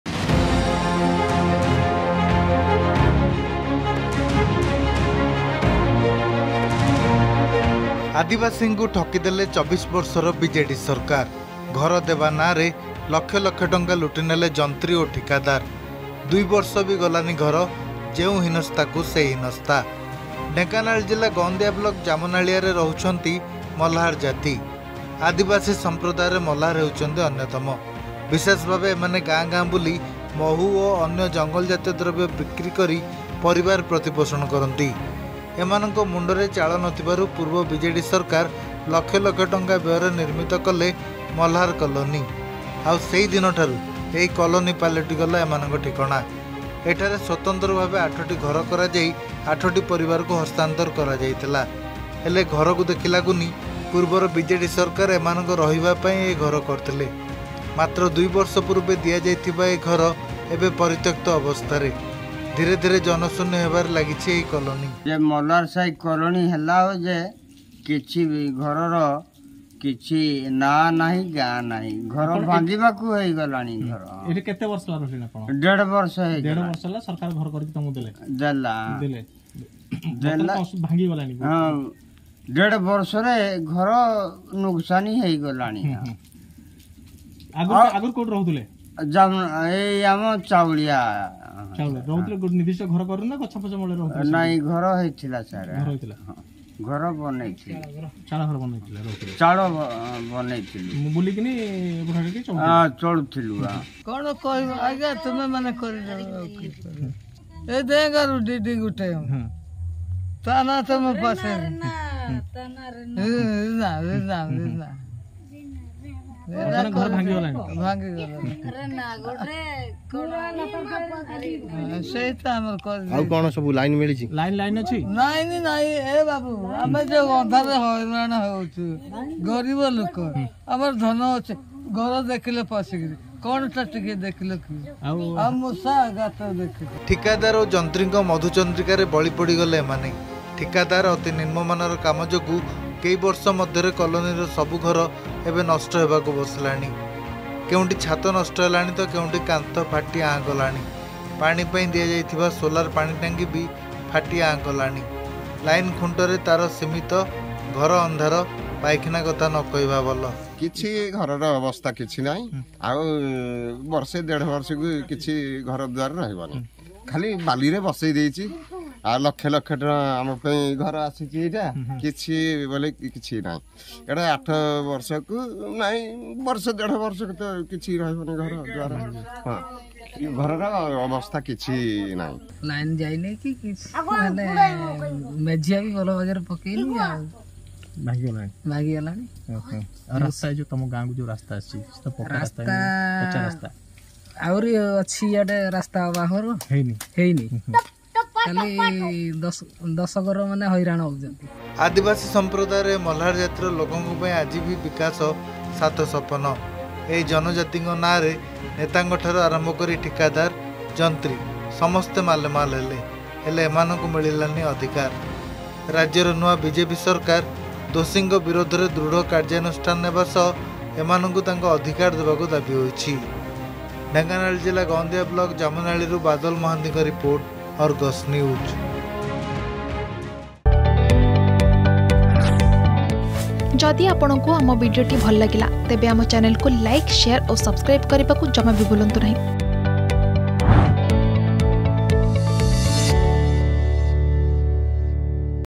ଆଦିବାସୀଙ୍କୁ ଠକିଦେଲେ ୨୪ ବର୍ଷର ବିଜେଡି ସରକାର || Dhenkanal || BJD
ଘର ଦେବା ନାଁରେ ଲକ୍ଷ ଲକ୍ଷ ଟଙ୍କା ଲୁଟିନେଲେ ଯନ୍ତ୍ରୀ ଓ ଠିକାଦାର। ୨ ବର୍ଷ ବି ଗଲାନି ଘର, ଯେଉଁ ହୀନସ୍ତାକୁ ସେଇ ହୀନସ୍ତ।ଏ ଦୃଶ୍ୟ ହେଉଛି ଢେଙ୍କାନାଳ ଜିଲ୍ଲା ଗଁନ୍ଦିଆ ବ୍ଲକ ଜାମୁନାଳୀଆର।